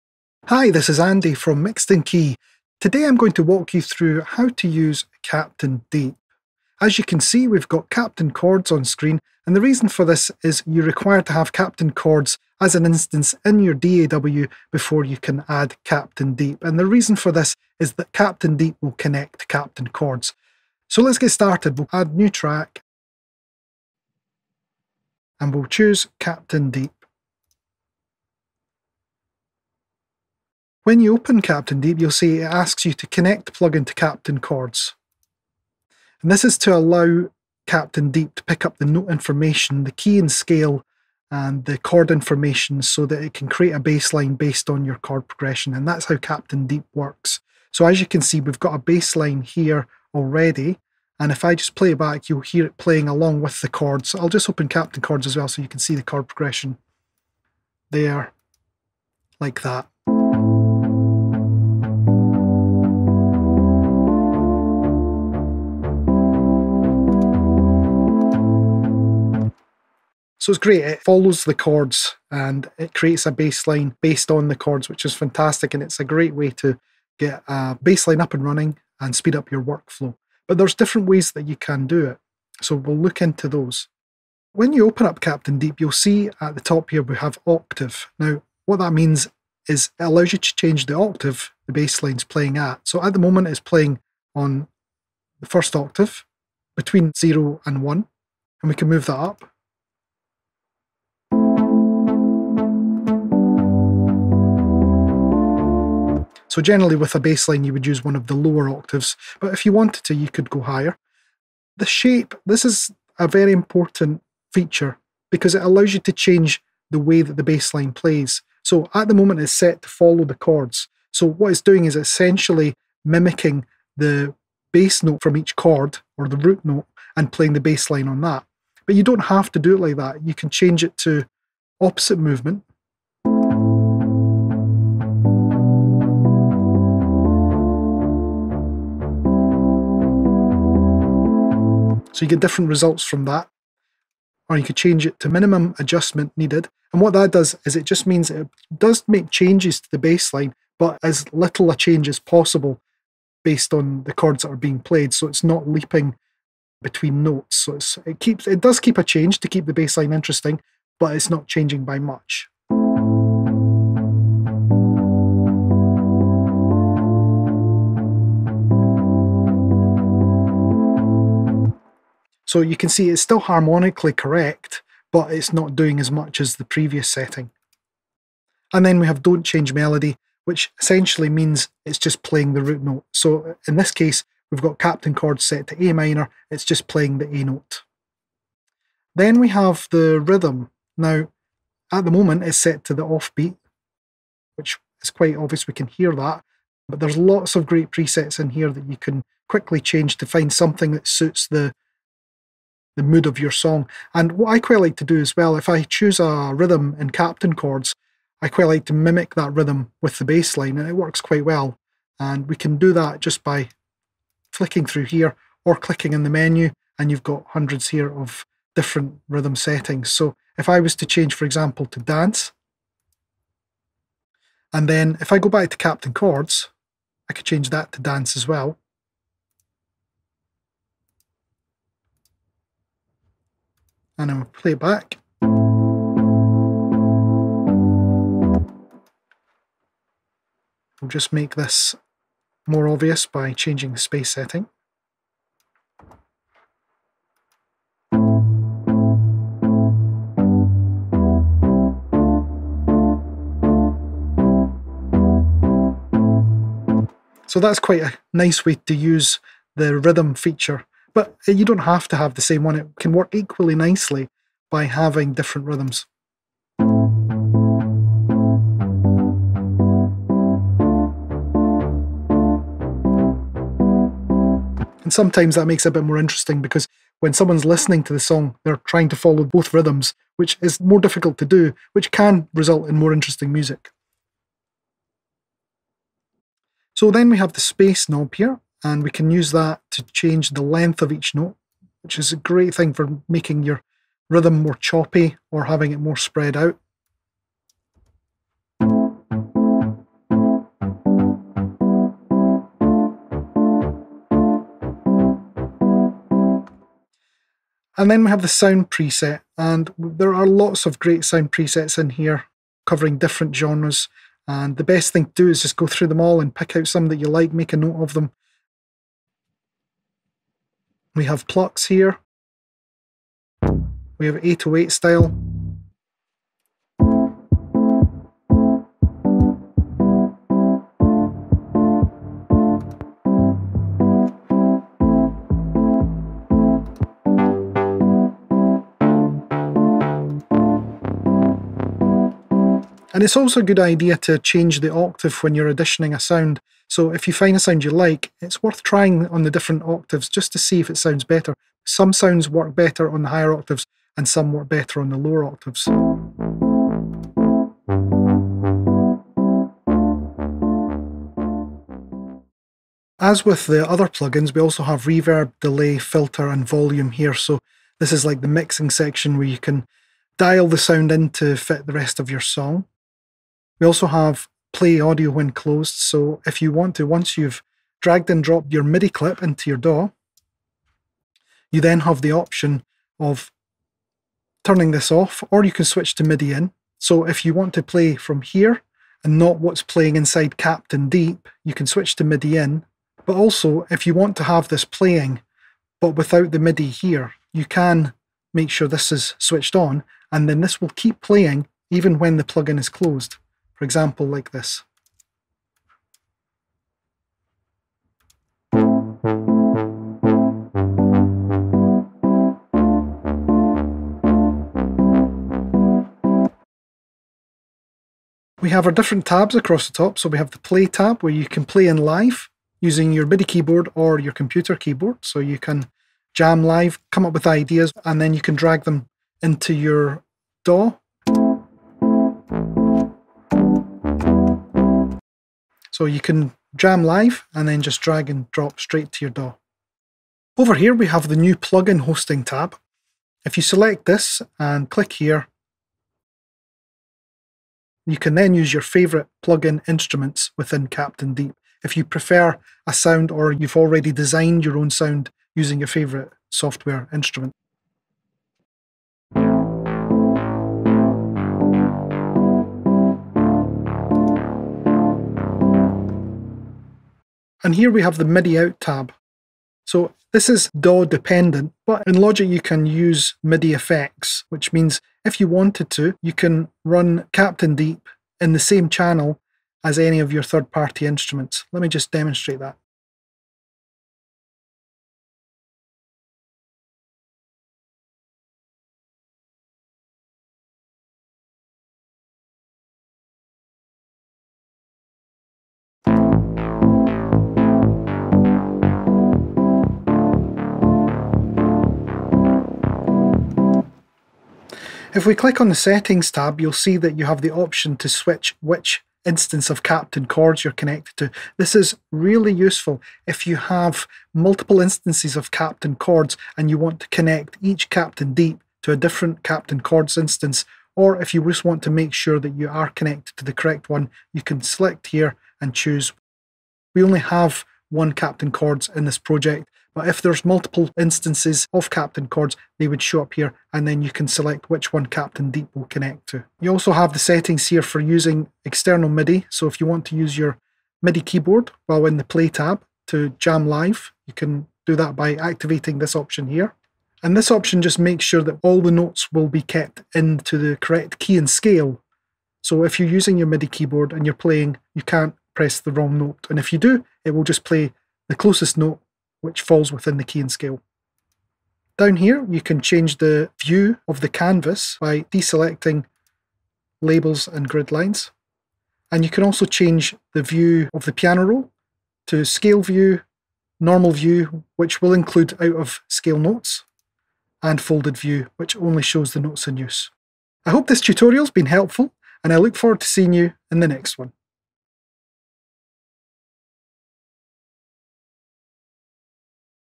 Captain Deep 5 Full Walkthrough/Captain Plugins Tutorial/ Bassline Generator
captainplugins.com - Take an in-depth look at all the features available on our Captain Deep plugin.

Don´t have a license yet? Buy your Captain Plugins Bundle today at captainplugins.com